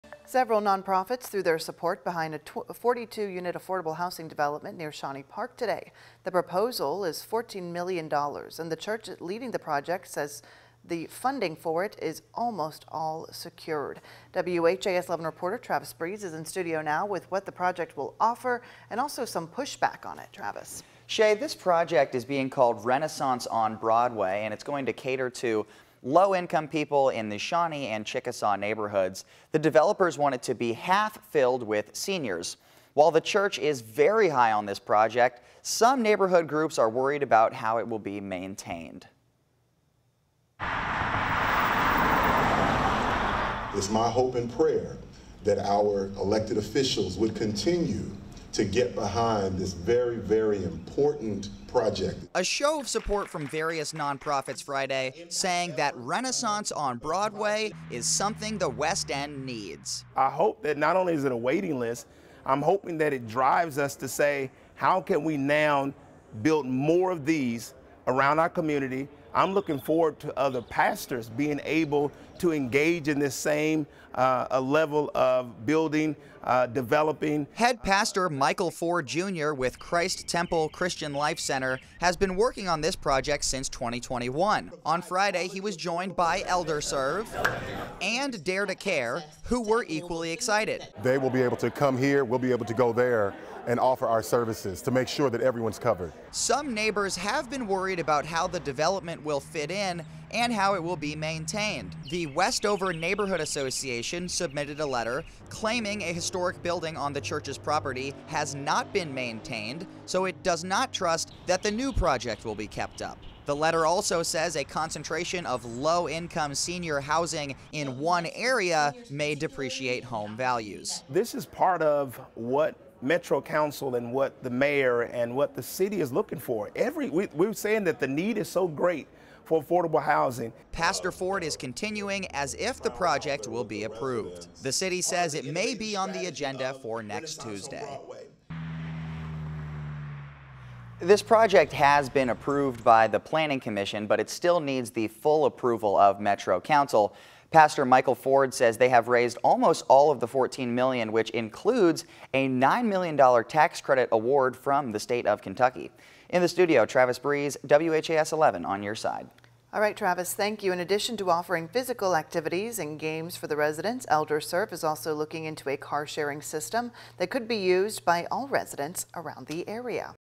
ElderServe joins Chickasaw affordable housing project, as some critics remain
Various groups such as ElderServe and Dare to Care have joined the project, but people living nearby question how well the building will be maintained.